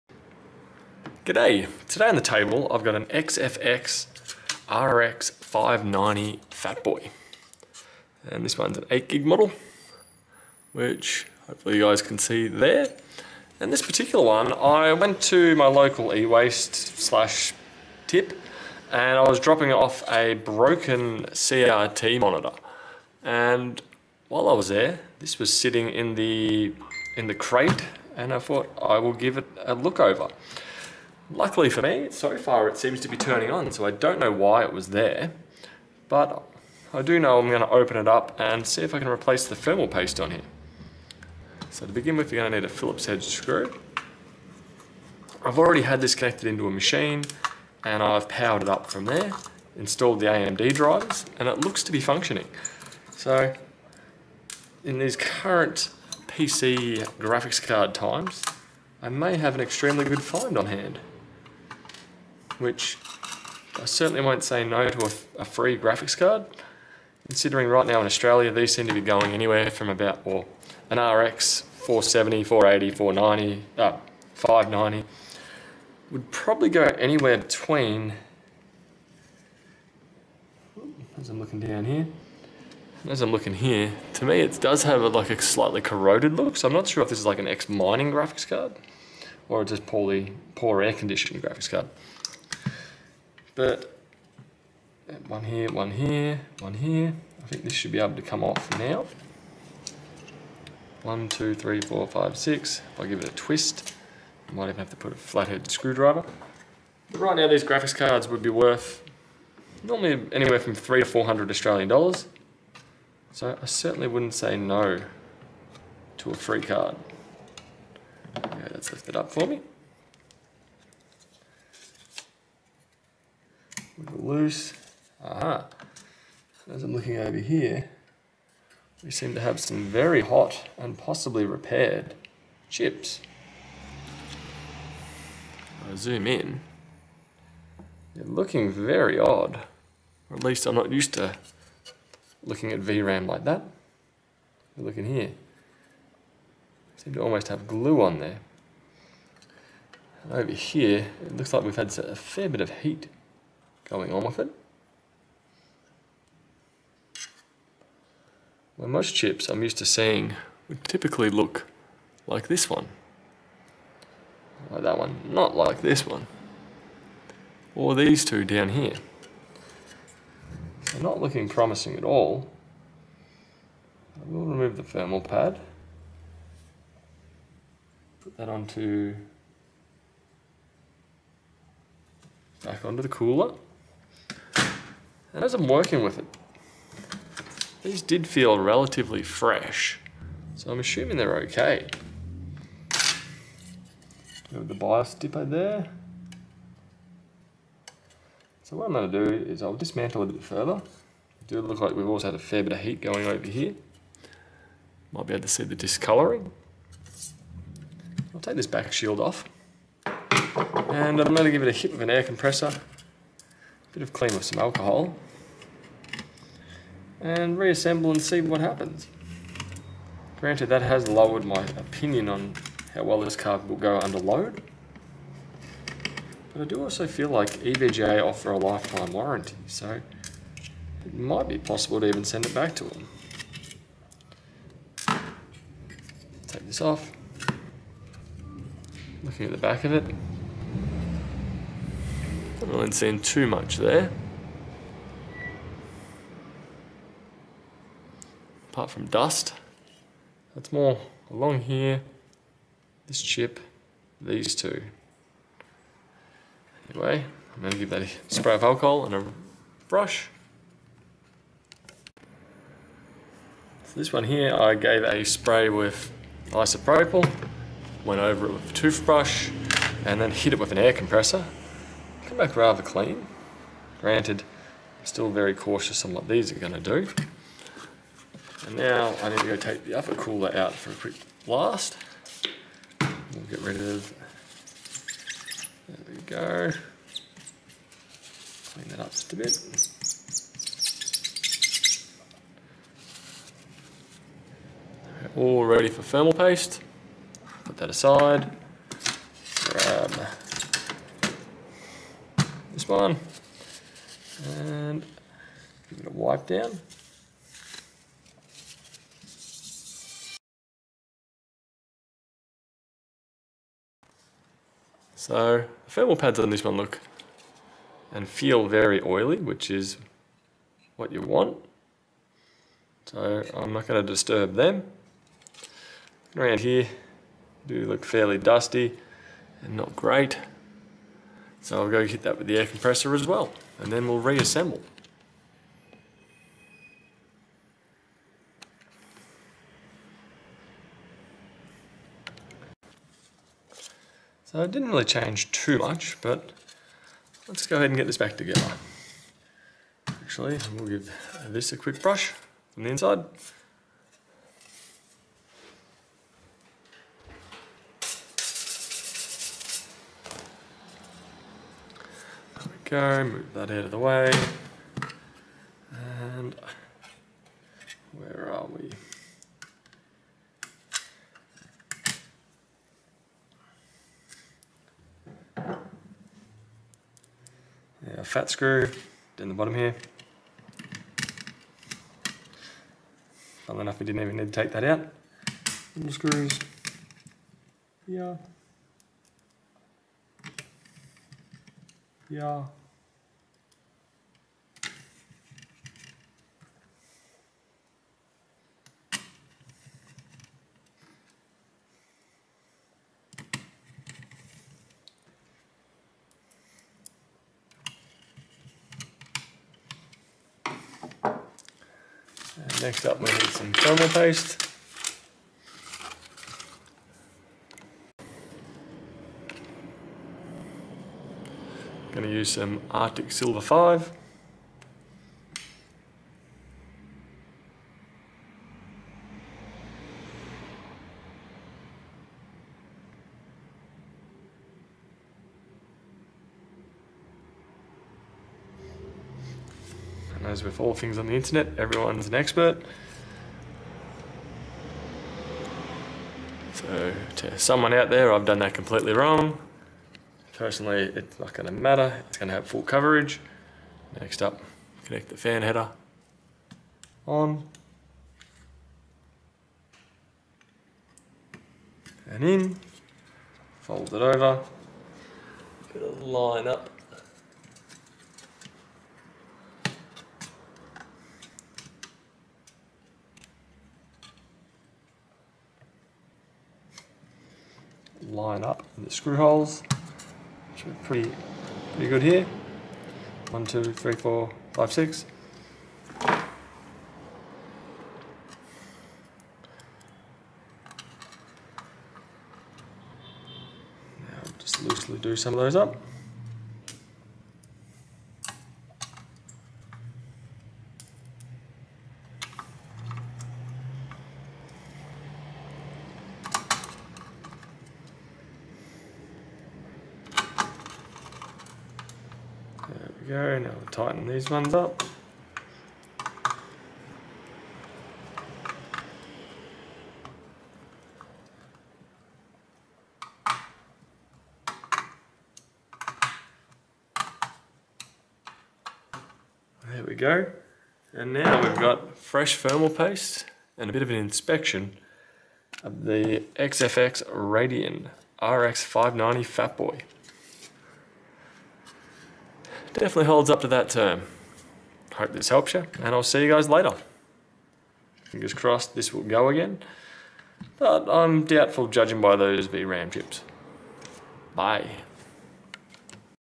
Trash Find!, XFX RX 590 Fatboy Thermal Replacement
In this video im doing a teardown of an XFX RX 590 that i found at a local ewaste recycle center when i was dropping off an Old CRT

Lets check it out and get it going again